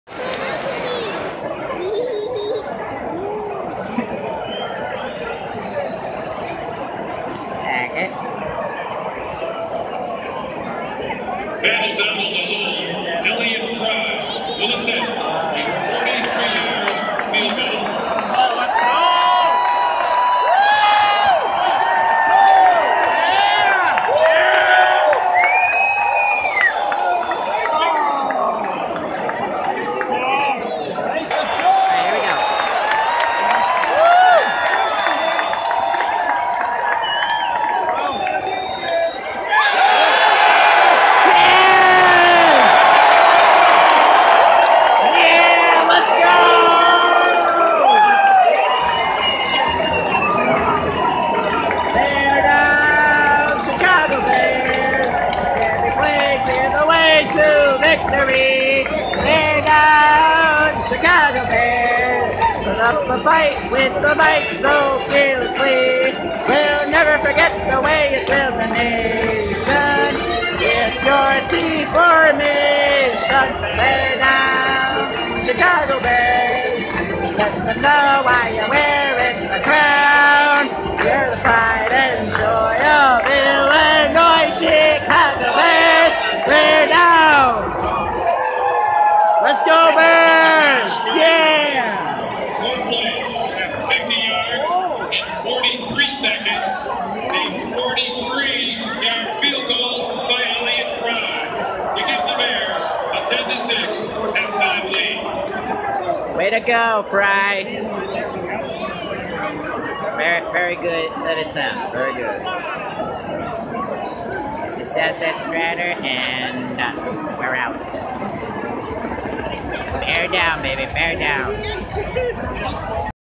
Bears vs Panthers preseason
Here's some footage of the Bears vs Panthers preseason game hope you enjoy and # go Bears